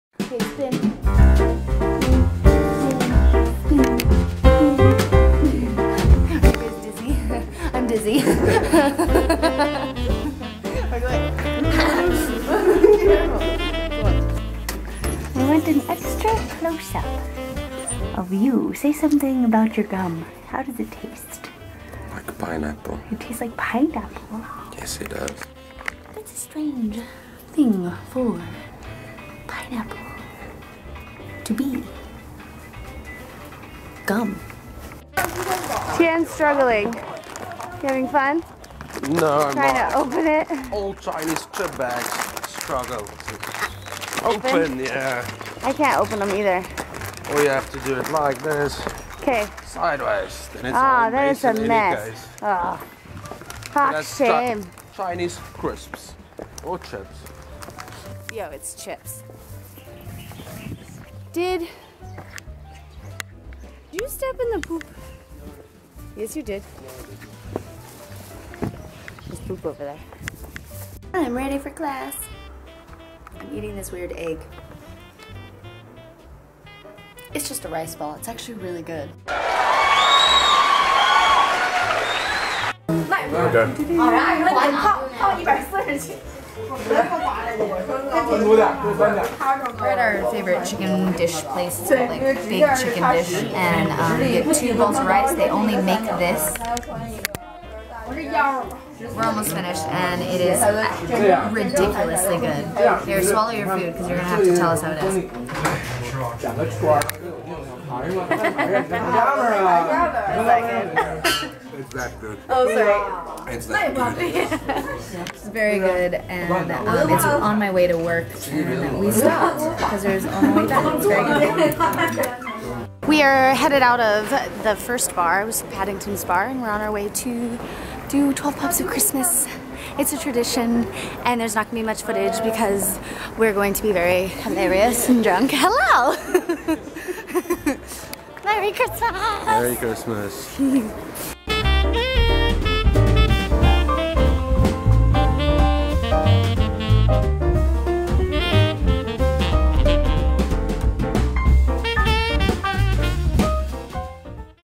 Big Chicken Dish | Vlogmas 10 | Wheelee's Travels
In this vlog, Wheelee and her beau run around Dalian eating all the food including the "Big Chicken Dish" restaurant (which only serves one type of dish and they do it RIGHT) It's stinking good, and because it's a Muslim place everything is clean. So you don't get sick. It's a must eat!
Then, they head to 12 pubs of Christmas, a Dalian (Irish) tradition.